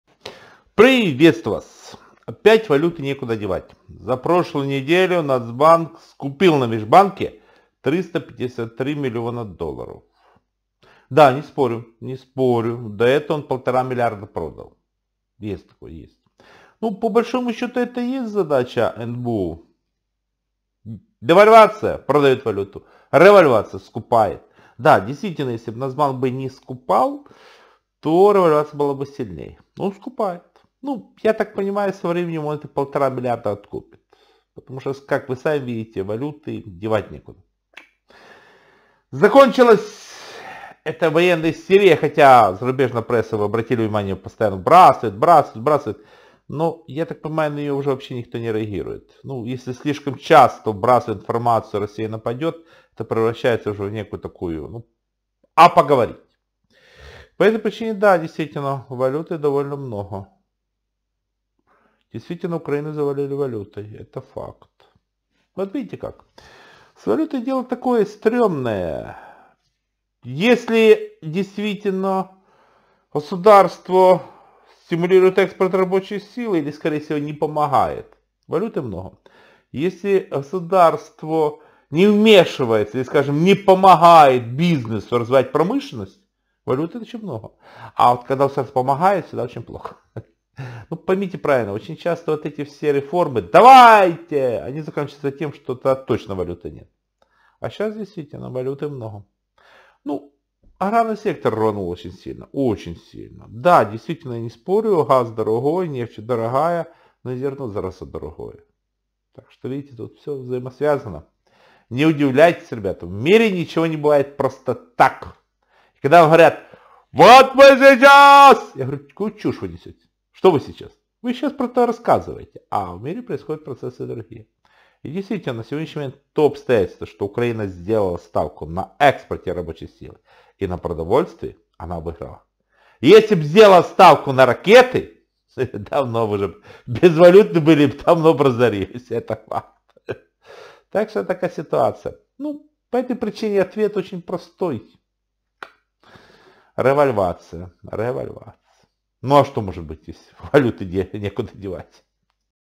Что ждет курс гривны завтра
Это хорошо, когда на валютном рынке нет стабильности. Ибо стабильный курс гривны навевает скуку и тогда уже всем грустно. А теперь есть повод поругаться и по скандалить, а за одно сделать прогноз курса гривны на ЗАВТРА